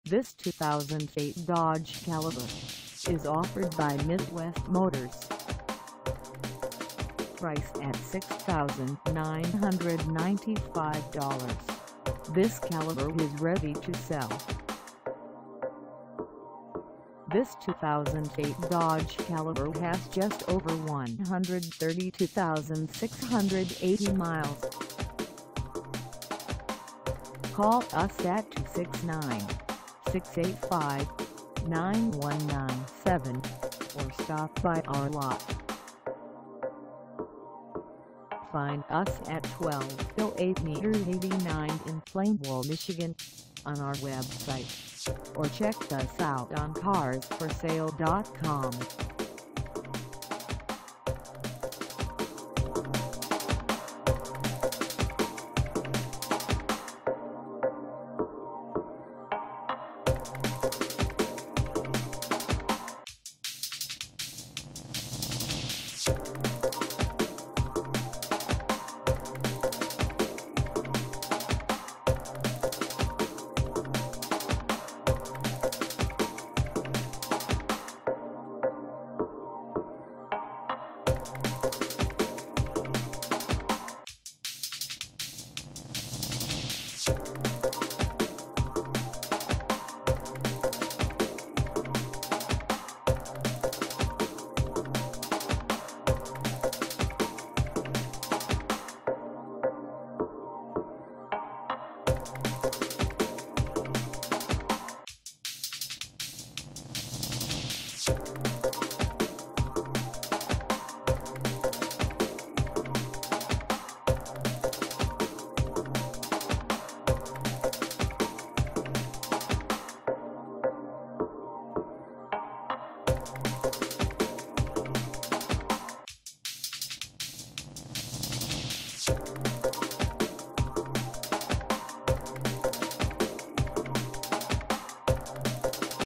2008 Dodge Caliber for sale in Plainwell, MI 49080 at the Mi
This 2008 Dodge Caliber is for sale in Plainwell, MI 49080 at Midwest Motors.
Engine: I4 2.0L I4
Transmission: CVT
Color: Dk. Red
Mileage: 132680
Contact Midwest Motors 1208 M 89 Plainwell, MI 49080 to test drive this 2008 Dodge Caliber  today.